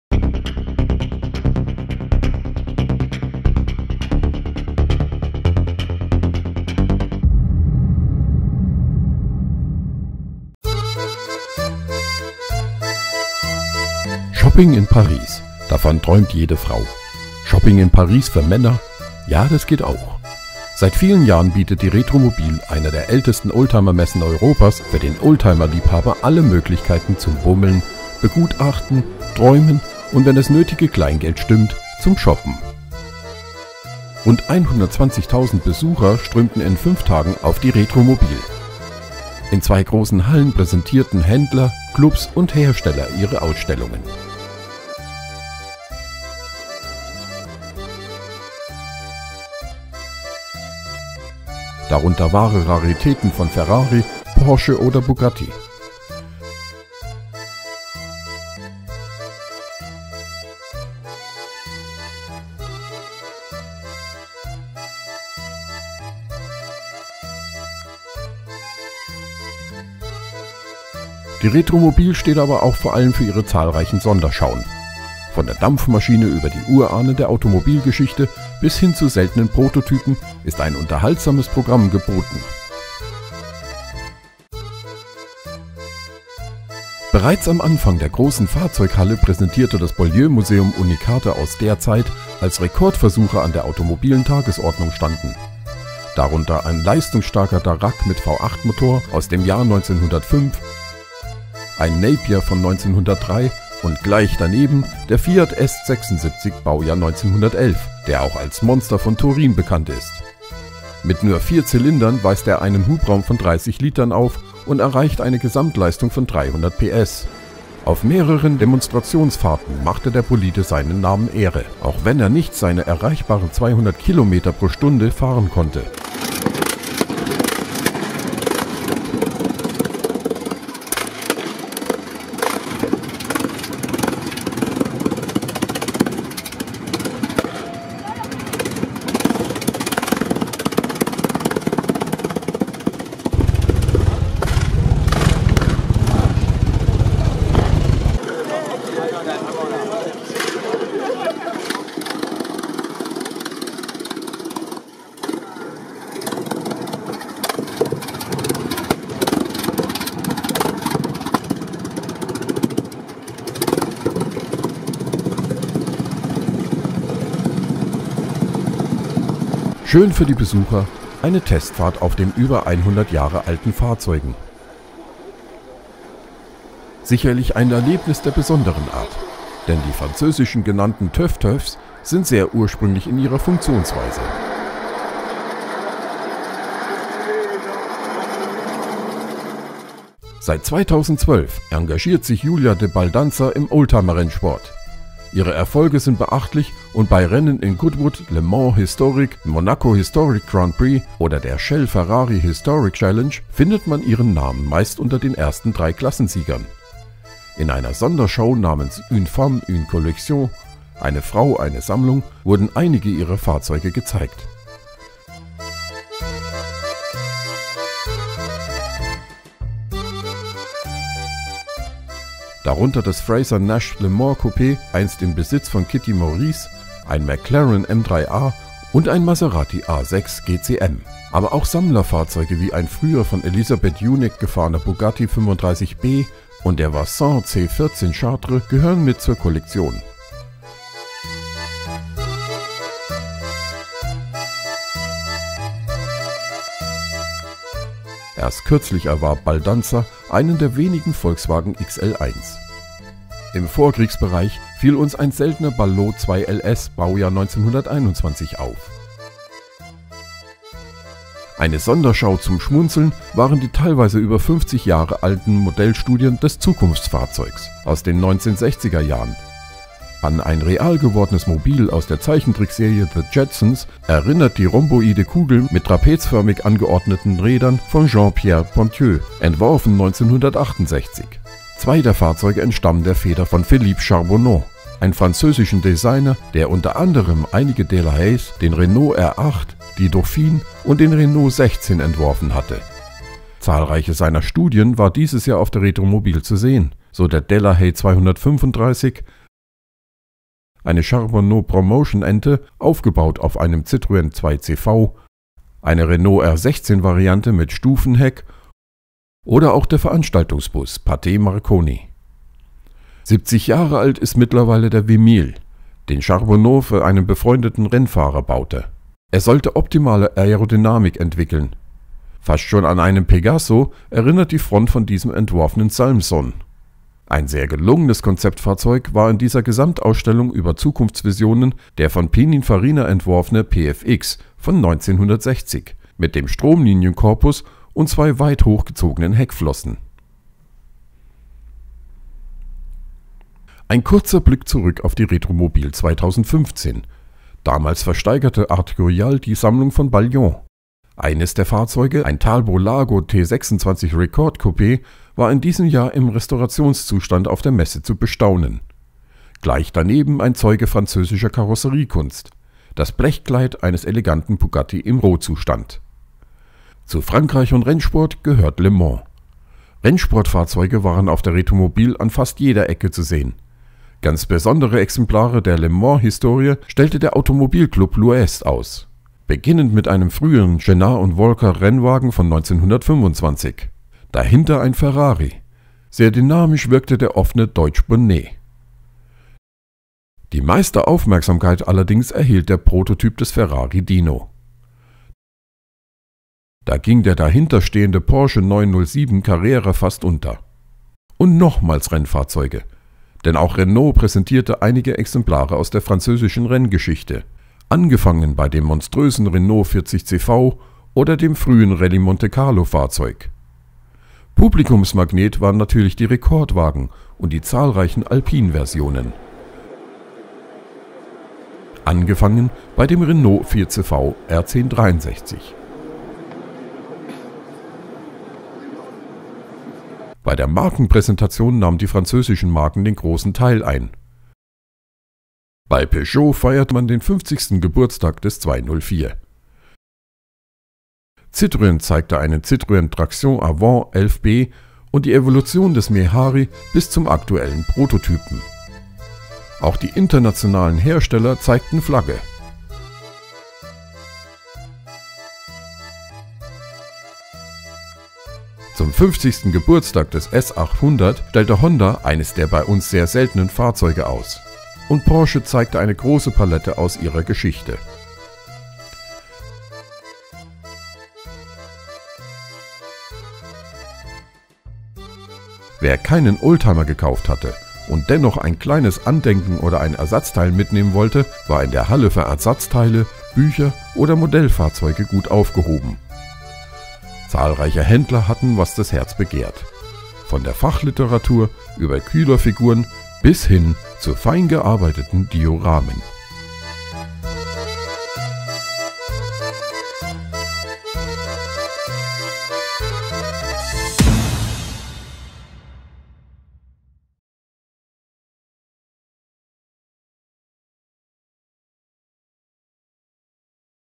ريتروموبيل باريس 2016موسم السيارات القديمة
توافد حوالي 120.000 زائر على Rétromobile في خمسة أيام.  قدم التجار والنوادي والمصنعون معارضهم في قاعتين كبيرتين.  بما في ذلك النوادر الحقيقية من فيراري أو بورش أو بوجاتي.  قبل كل شيء ، ومع ذلك ، فإن Rétromobile يقف على عروضه الخاصة العديدة.  تم تقديم برنامج ترفيهي ، من المحرك البخاري إلى أسلاف تاريخ السيارات إلى النماذج الأولية النادرة

Facebook Kamal maroc diag
Instagram the mechanic T CC Bk